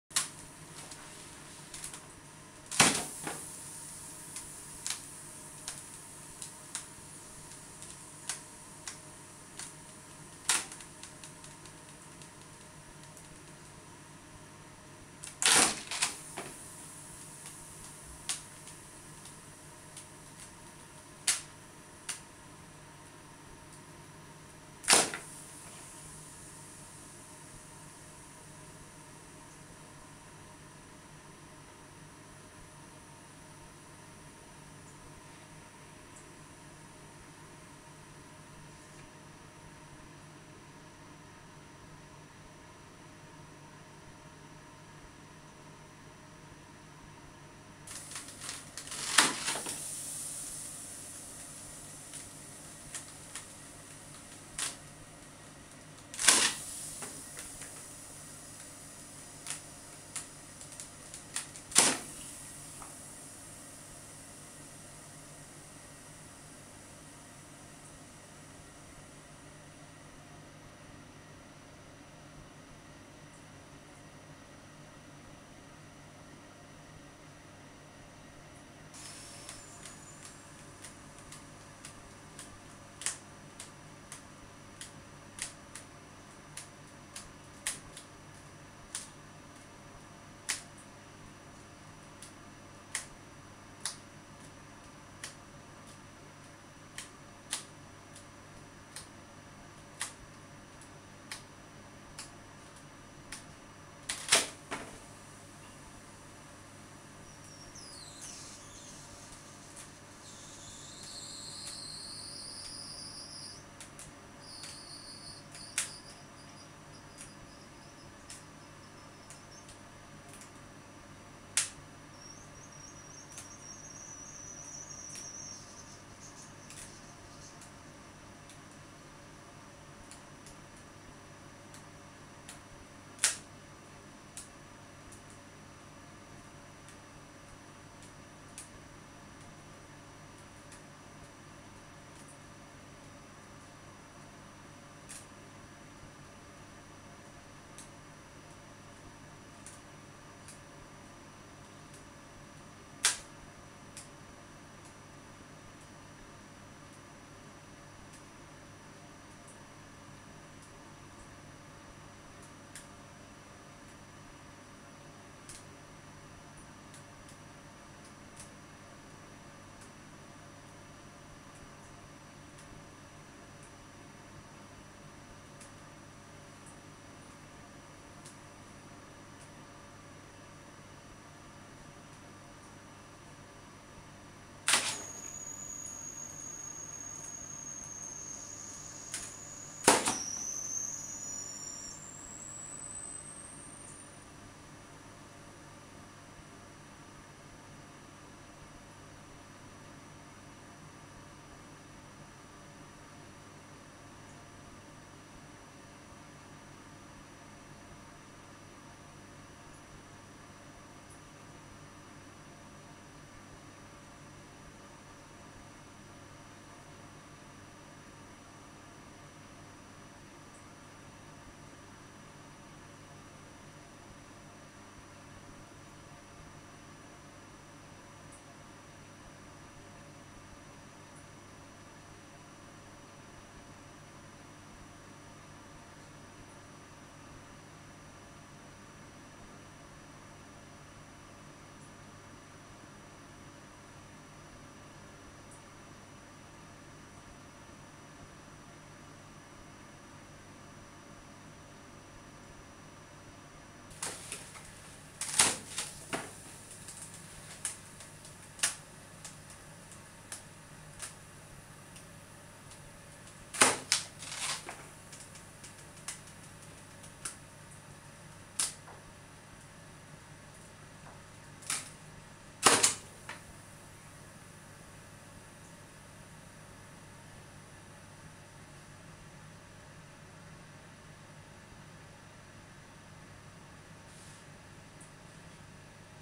dog-sausages
a balloon dog turns into a string of sausages